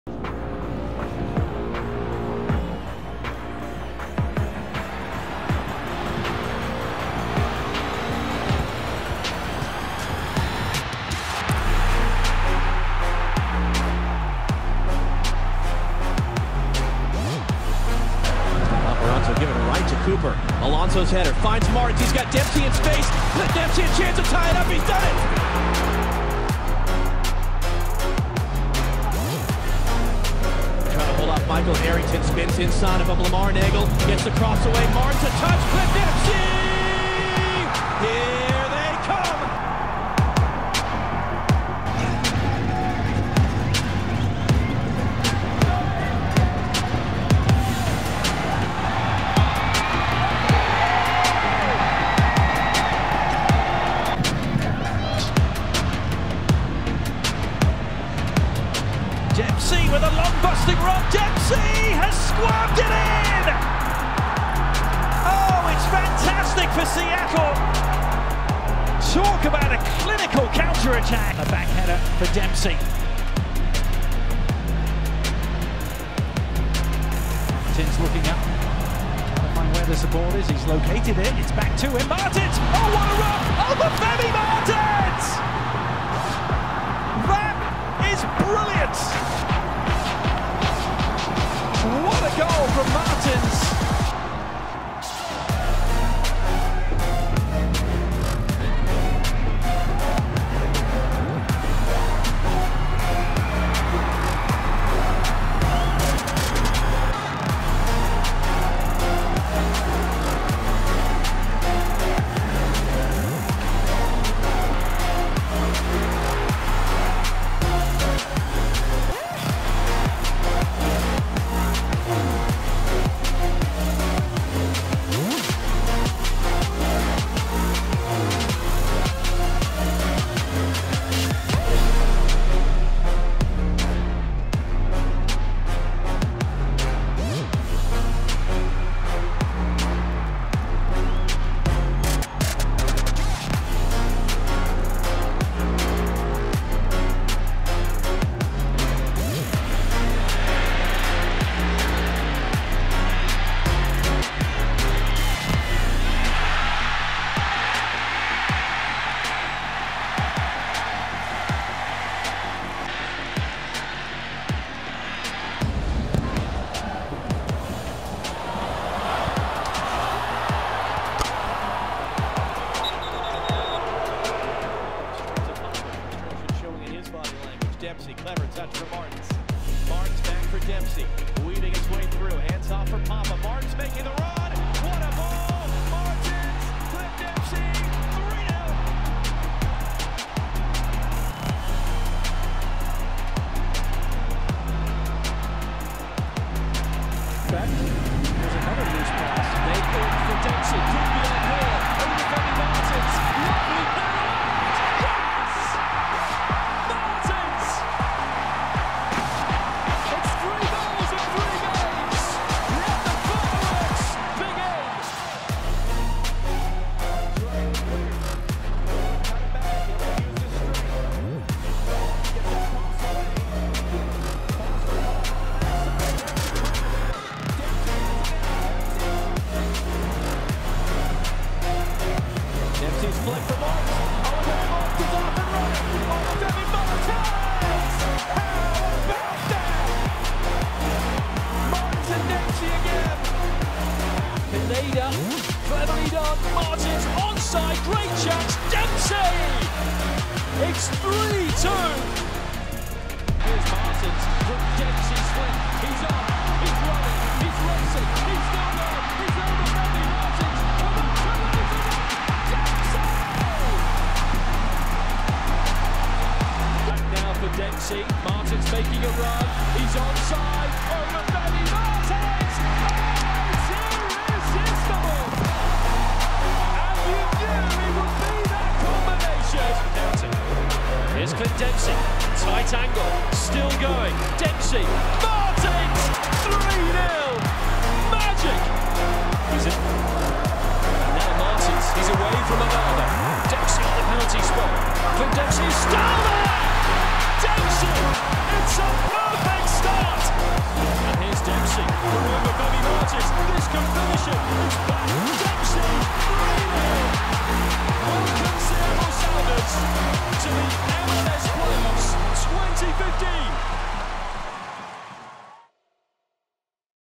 The Best of Clint Dempsey and Obafemi Martins with Sounders FC
Take a look back at Seattle Sounders FC forwards Clint Dempsey and Obafemi Martins' best linkups with the Rave Green.